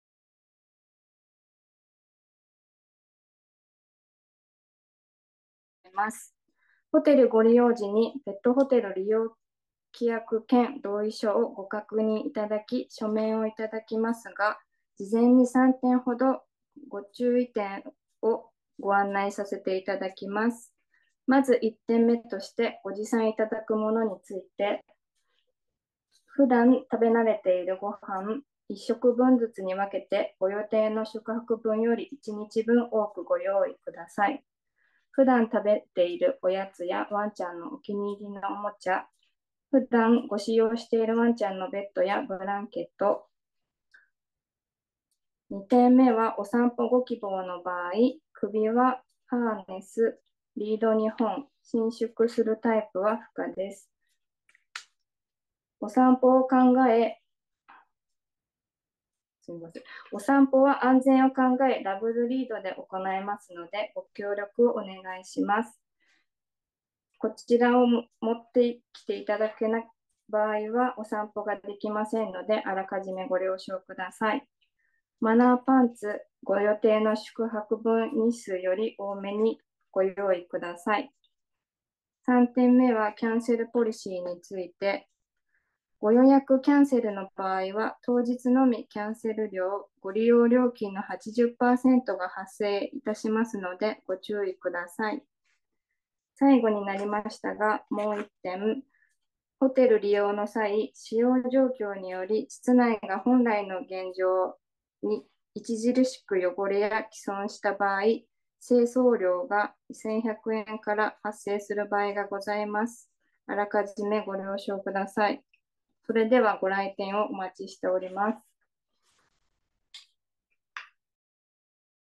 ペットホテル利用時の注意点
【必読❣ペットホテルごご予約のお客様へ】

『利用時の注意』を送信させていただきます。

必ずご確認いただいてからのご来店をお願いいたします🐶

引き続き皆様により良いサービスをお届けできるよう
スタッフ一同精進してまいりますので、ご理解のほどよろしくお願い申し上げます。